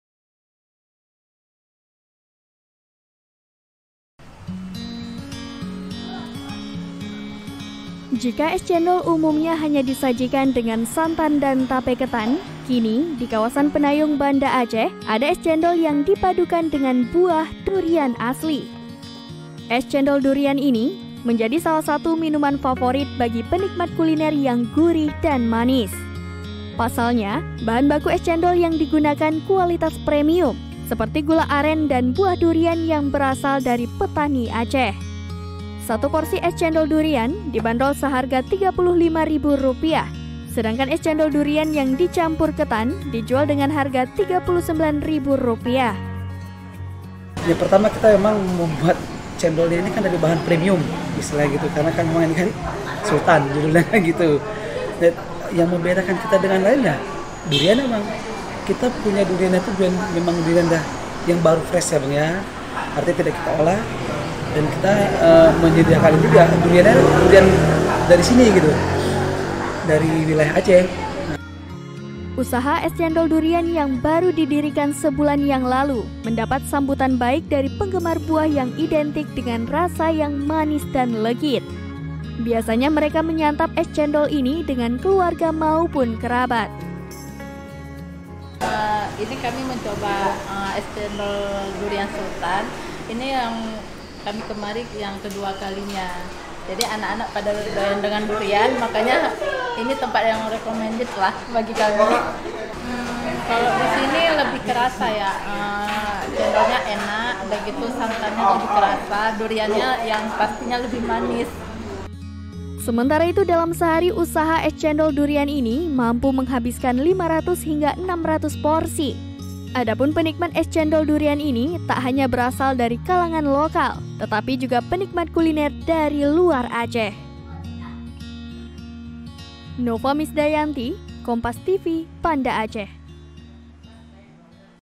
Nikmatnya Es Cendol Durian Asli Aceh
Cat Tembok Andalan Sejak 1960 dengan Garansi Hasil Bagus. Bagusnya Vinilex dari Nippon Paint. Cek di sini

BANDA ACEH, KOMPAS.TV - Jika es cendol umumnya hanya disajikan dengan santan dan tape ketan, kini di kawasan Peunayong, Banda Aceh ada es cendol yang dipadukan dengan buah durian asli.

Es cendol durian ini menjadi salah satu minuman favorit bagi penikmat kuliner yang gurih dan manis. Pasalnya bahan baku es cendol yang digunakan kualitas premium, seperti gula aren dan buah durian yang berasal dari petani Aceh.

Baca Juga Tiga Maskapai Siap Layani Penerbangan Internasional dari Aceh di

Satu porsi es cendol durian dibandrol seharga 35 ribu rupiah sedangkan es cendol durian yang dicampur ketan dijual dengan harga 39 ribu rupiah.

Usaha es cendol durian yang baru didirikan sebulan yang lalu mendapat sambutan baik dari penggemar buah yang identik dengan rasa yang manis dan legit. Biasanya mereka menyantap es cendol ini dengan keluarga maupun kerabat.

Sementara itu dalam sehari usaha es cendol durian ini mampu menghabiskan 500 hingga 600 porsi. Adapun penikmat es cendol durian ini tak hanya berasal dari kalangan lokal tetapi juga penikmat kuliner dari luar Aceh.